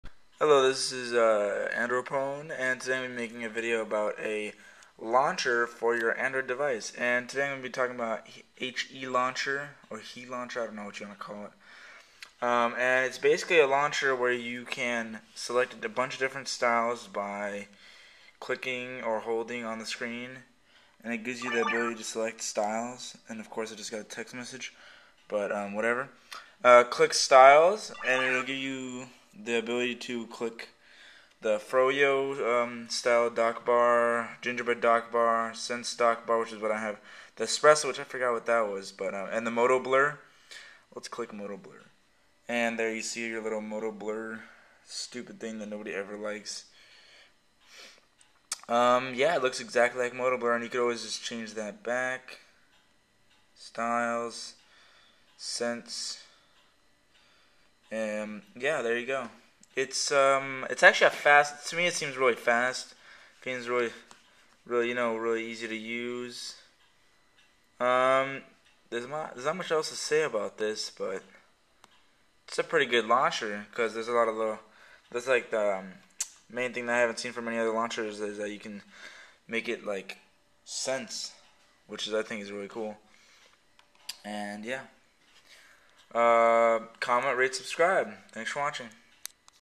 Android Add On: HE Launcher
a very good fast launcher it has ability to change the look of the dock